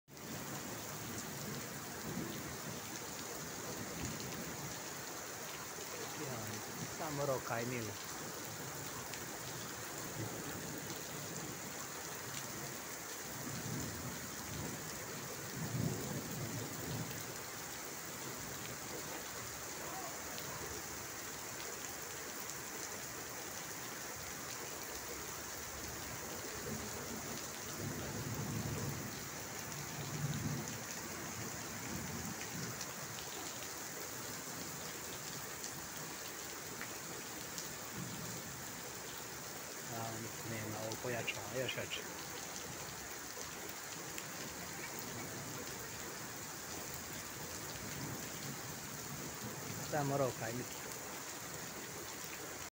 Nevrijeme u porto-Novi (čekajući prevoz do kuće) 01.Jul 2023.
Hvalilo mi je 10' koliko mi je trebalo od porta do kuce da bih izbjegao kisu